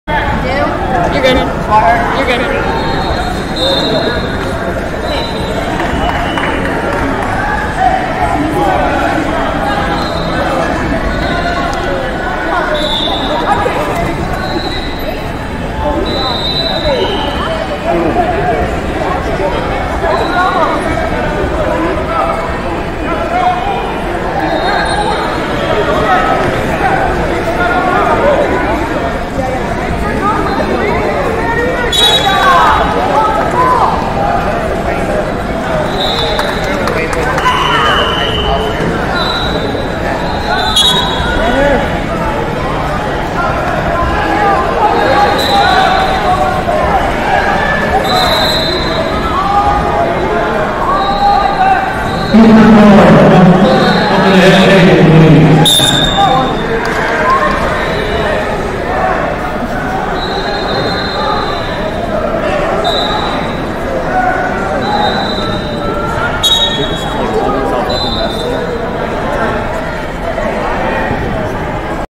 138 Lbs Quarterfinal - Kelcey Dew, OH Vs Danni Swihart, MI A0c0
2022 Central Regional Championships - 138 Lbs Quarterfinal - Kelcey Dew, OH Vs Danni Swihart, MI A0c0